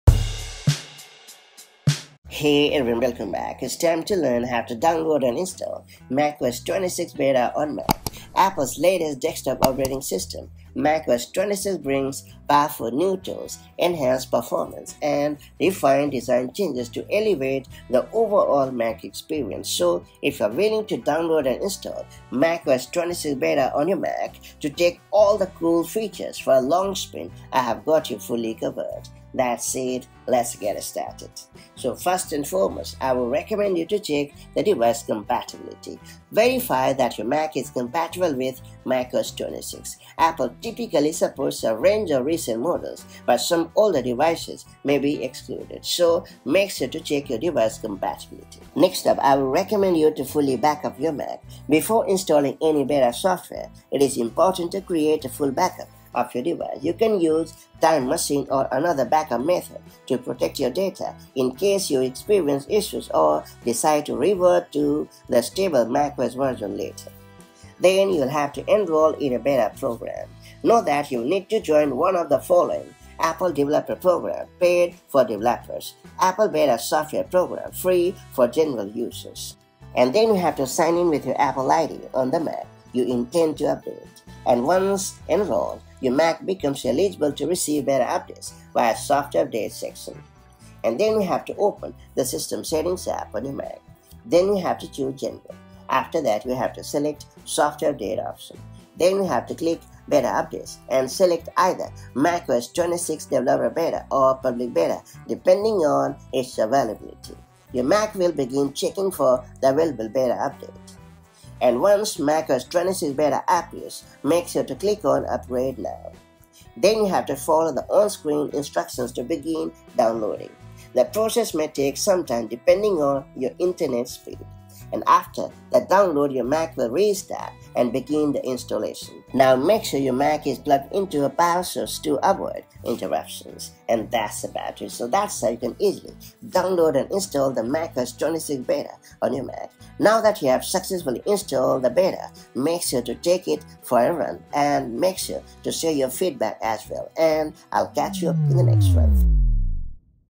How to Download & Install macOS Tahoe 26 - SECURELY!
Ready to explore macOS Tahoe 26 but want to do it the safe way? In this video, we guide you step-by-step on how to securely download and install macOS 26 (Tahoe) on your Mac without risking your data or system stability.